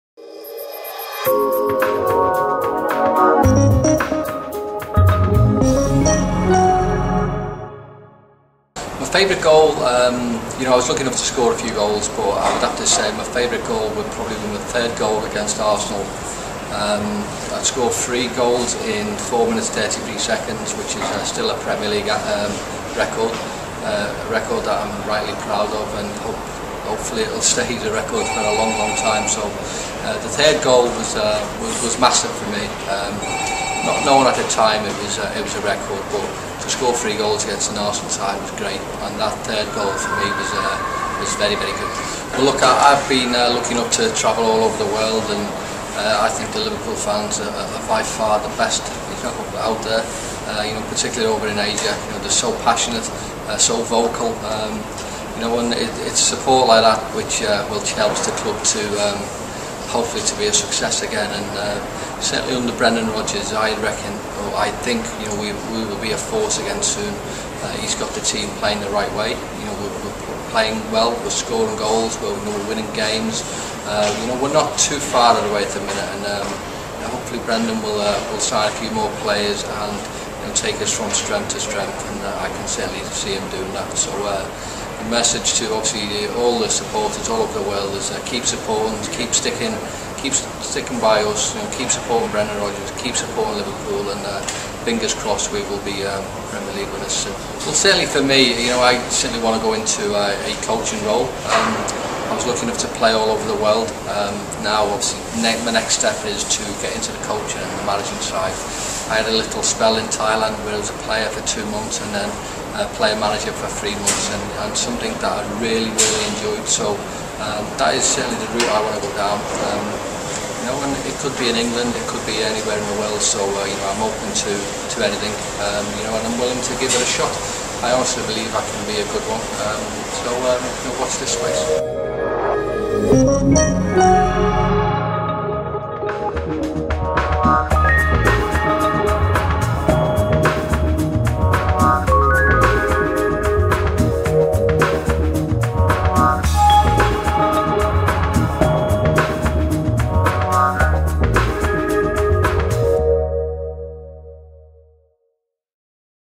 Interview with Robbie Fowler at Go! Partner Conference 2014
Go! Partner Conference guest speaker Robbie Fowler shares leadership lessons from his years playing with Liverpool Football Club. The Go! Partner Conference, which was held in Hanoi from 13 to 15 January 2014, provided a great opportunity for Siemens PLM to reach out to its customers and partners.